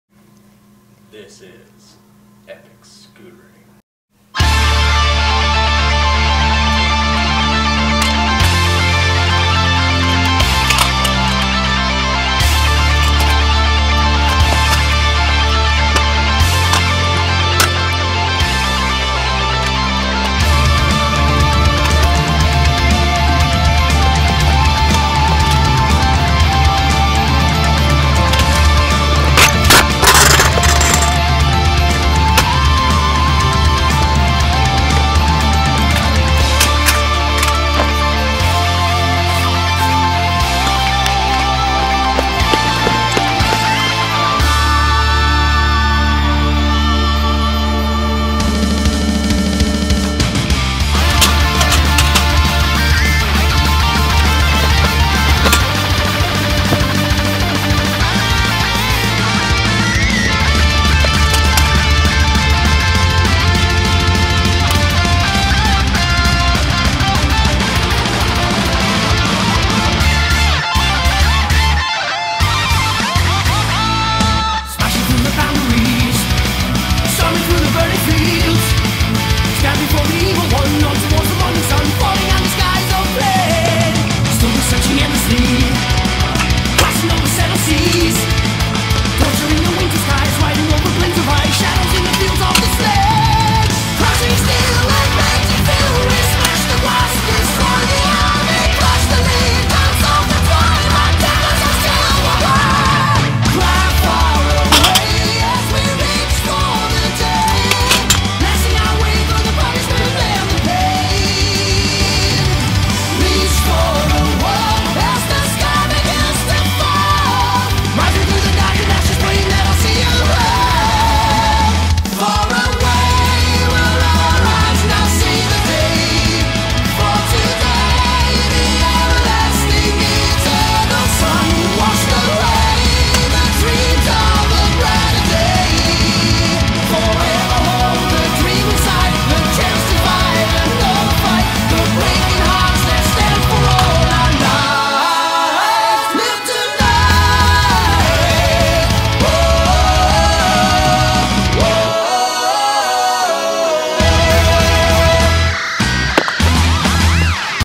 Epic Scootering
Two of the best amateur scooterers of our time.